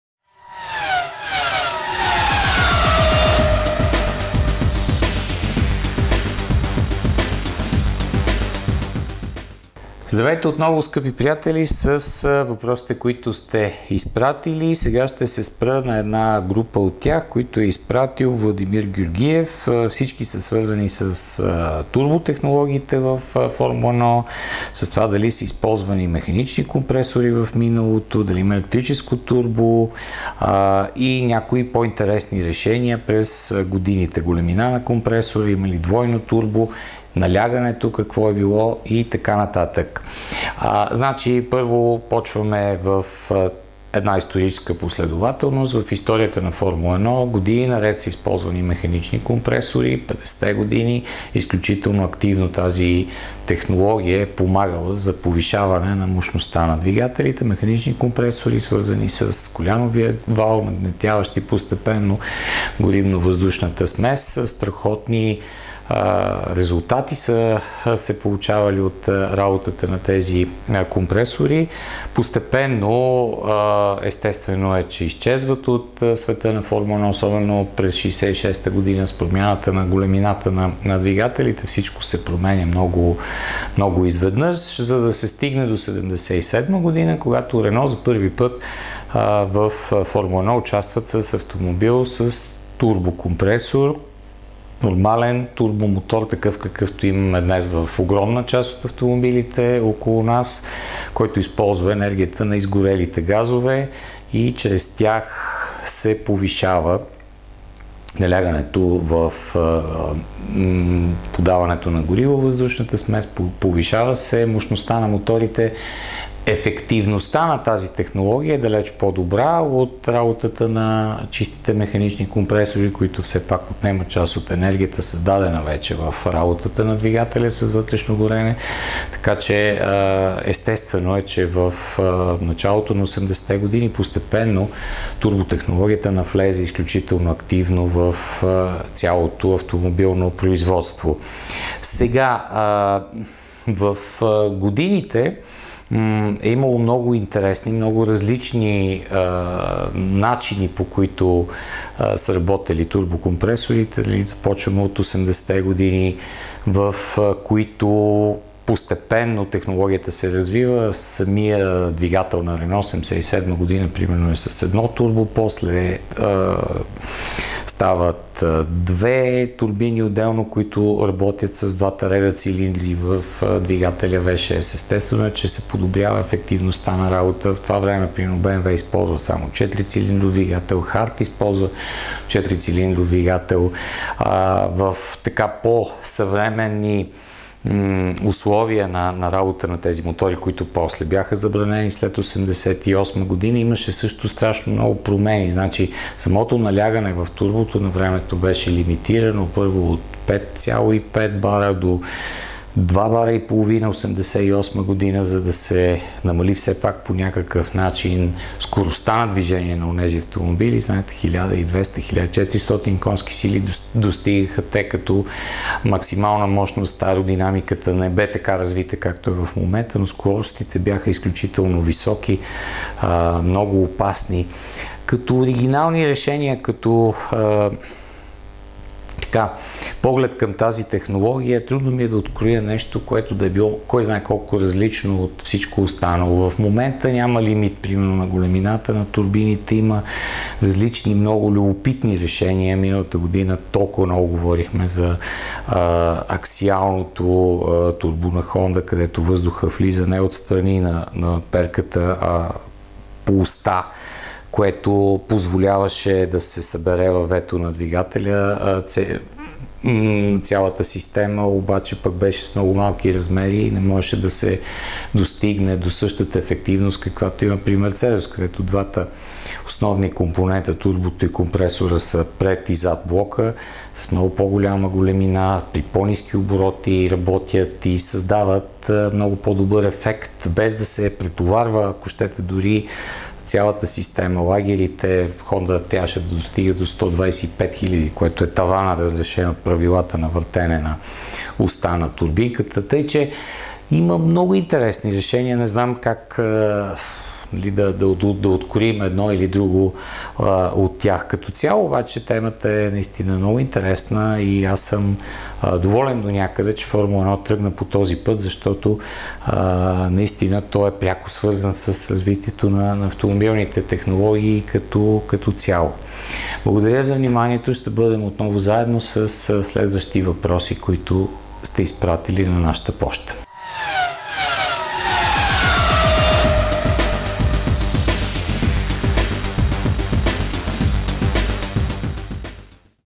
Турбокомпресорите във Формула 1
Темата в епизода са турбокомпресорите. Владимир Георгиев задава серия въпроси по темата:

1.Предимствата и недостатъците на двете основни турбо технологии/ механичен компресор и турбо, задвижвано от изгорелите газове/? Бих се радвал да добавите и няколко думи относно електрическото турбо.

2.Използван ли е някога механичен компресор във Формула 1?

3.Кои са най-смелите турбо решения, използвани през годините във Формула 1? Например големина на компресора, двойно турбо, налягане и т.н.